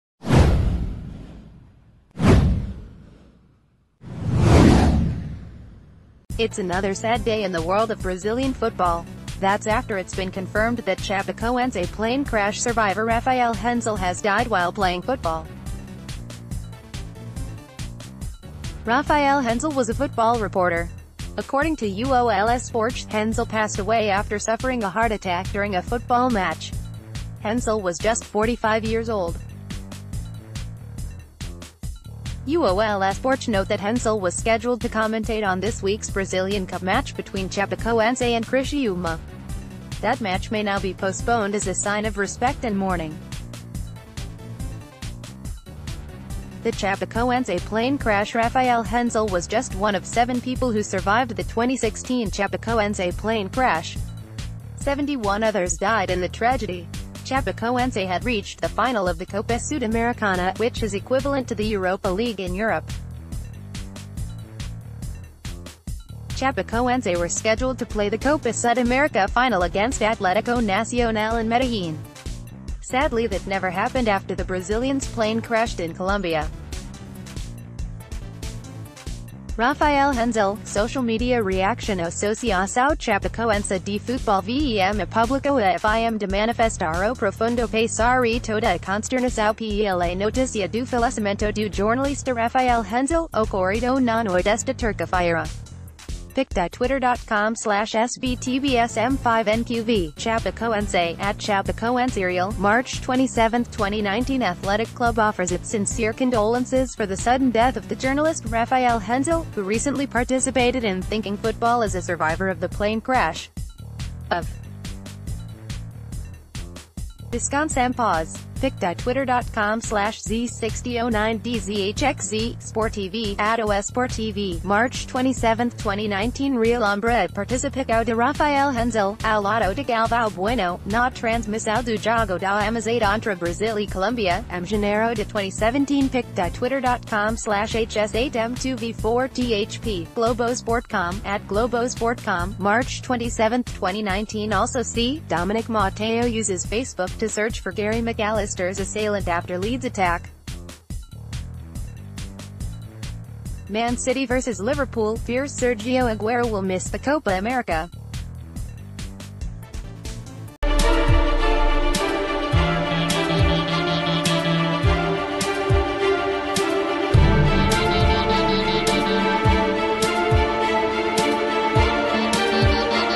Chapecoense plane crash survivor Rafael Henzel dies playing football
It’s another sad day in the world of Brazilian football. That’s after it’s been confirmed that Chapecoense plane crash survivor Rafael Henzel has died while playing football. Rafael Henzel was a football reporter. According to UOL Esporte, Henzel passed away after suffering a heart attack during a football match. Henzel was just 45-years-old. UOL Esporte note that Henzel was scheduled to commentate on this week’s Brazilian Cup match between Chapecoense and Criciúma. That match may now be postpon...